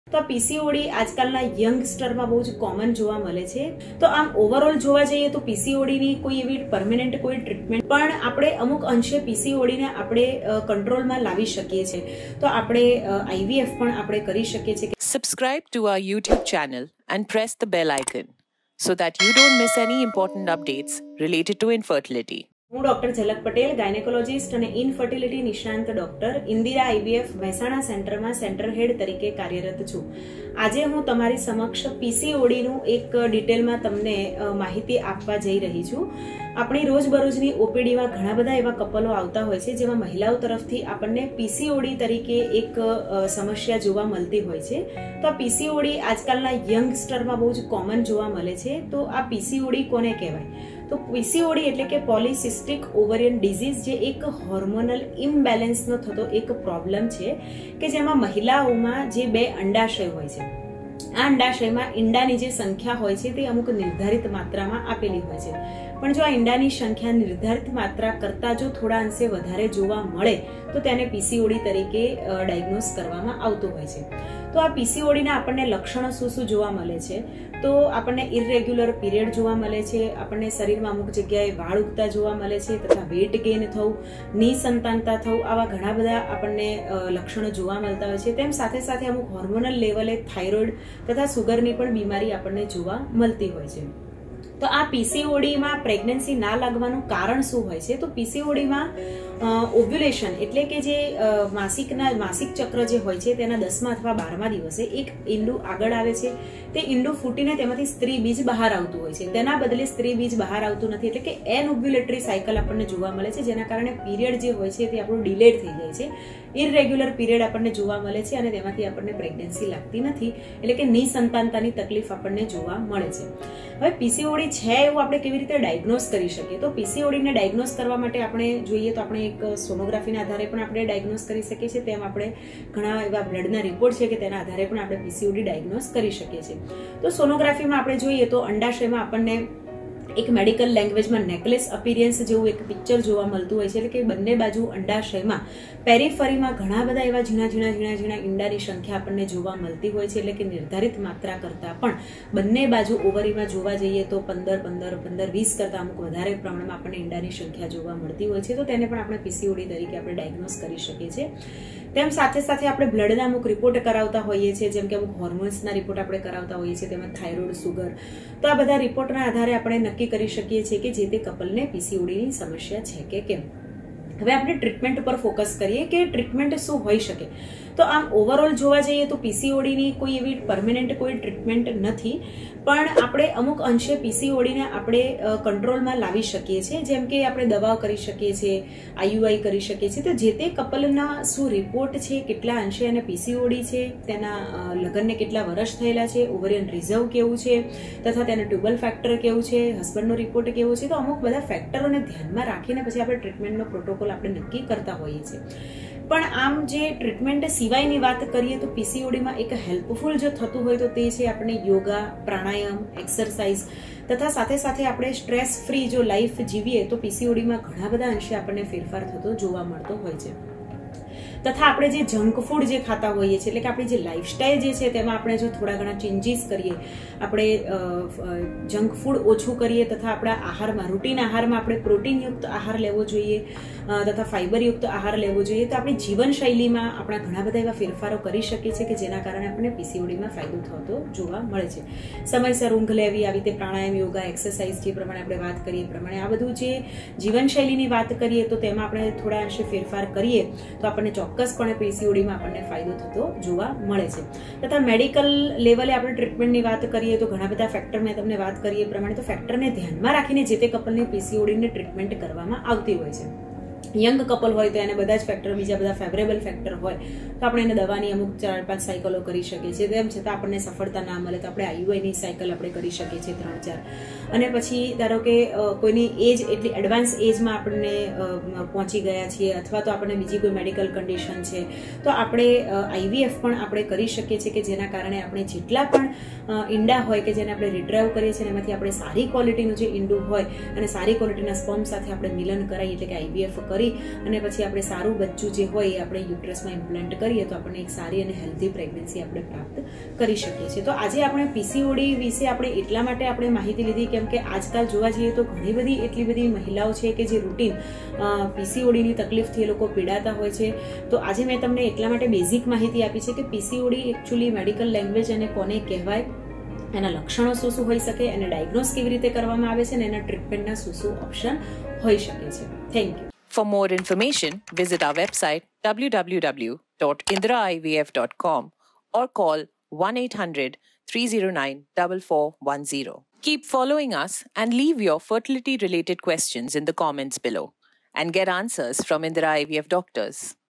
PCOD અને વંધ્યત્વ: તેઓ કેવી રીતે એકબીજા સાથે સંબંધિત છે? લક્ષણો અને વ્યવસ્થાપન સુચન જાણો
ડૉક્ટર ઝલક પટેલ, ગાયનેકોલોજિસ્ટ અને વંધ્યત્વ નિષ્ણાત, અને ઈન્દિરા IVF મહેસાણા (ગુજરાત)ના સેન્ટર હેડ સાથે જોડાઓ. તેણી આ માહિતીપ્રદ વિડિયોમાં PCOD (પોલીસીસ્ટિક અંડાશયના રોગ) વિશે વિગતવાર સમજૂતી આપે છે. PCODનાં લક્ષણો, વંધ્યત્વના કારણો, નિદાન પદ્ધતિઓ અને સારવારના વિકલ્પો શોધો. જો તમે અથવા તમે જાણો છો તેવી કોઈ વ્યક્તિ PCOD થી પ્રભાવિત છે, તો આ વિડિયો જોવો જ જોઈએ. PCOD સંબંધિત વંધ્યત્વ સમસ્યાઓ માટે ઉકેલ શોધવા તરફ પ્રથમ પગલું ભરો, આજે જ તમારા નજીકના ઇન્દિરા IVF કેન્દ્રની મુલાકાત લો. અમારા નિષ્ણાતો સાથે મફત પરામર્શ બુક કરવા માટે, કૉલ કરો: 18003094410